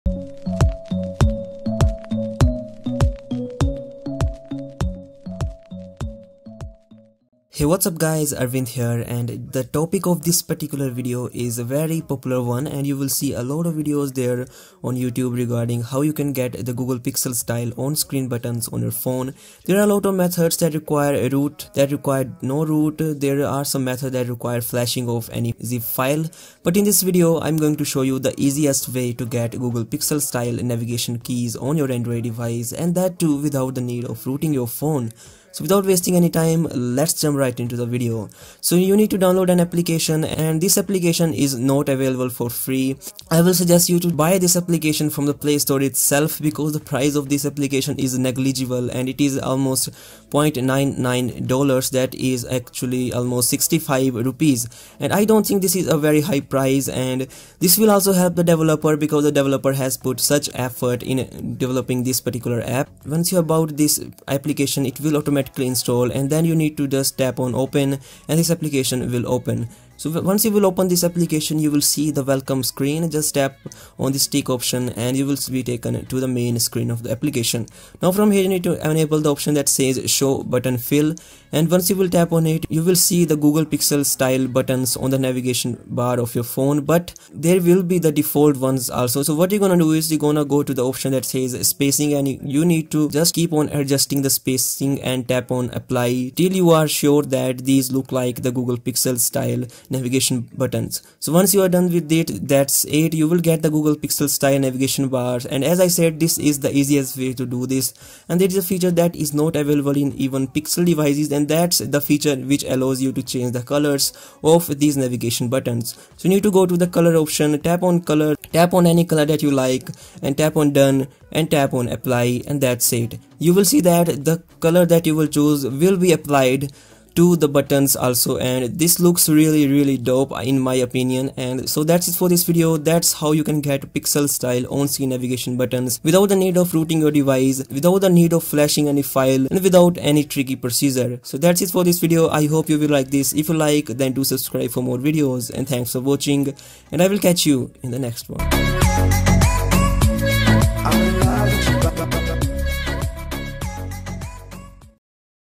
[NO ROOT] How To Get Google Pixel NavBar On Any Android Phone | 2017
Hey What is up guys, in this video I will show you How to Get Google Pixel NavBar without Root on Any Android Phone running Android 5.0 and above. There are a ton of videos on YouTube, and most of them require ROOT ACCESS. There are some that don't require root, but the procedure might be confusing for you. In this video, I will show you how to get Pixel Navigation bar without any Root Access and without any complicated Procedure.

Google's own devices have always been the first to get new Android features—but unlike the Nexus series, this year's Pixel phones have a handful of exclusive tweaks that were never intended to trickle down to other devices once the newer Android version rolled out to them. These Pixel exclusives include the Google Assistant, a new launcher, and, of course, a unique set of on-screen navigation buttons.

The development team at Dunrite Apps has stepped in and provided a clever workaround for non-rooted users; Their latest app simply colors in your existing on-screen buttons to make them look almost exactly like the Pixel's.

➡️️ NOTE: First up, you should know that this app only works right if you have on-screen navigation buttons, so this isn't for devices that have hardware or capacitive keys. In addition to that, it's only recommended if your phone has the newer-style triangle-circle-square buttons!

Please BUY THE APP from the Play Store because it will help the developers. We usually try to find cracks of PAID APPS even when the app costs less than the price of a Burger. The developers have spent weeks and even months developing the app so I will recommend buying it to respect their hard work.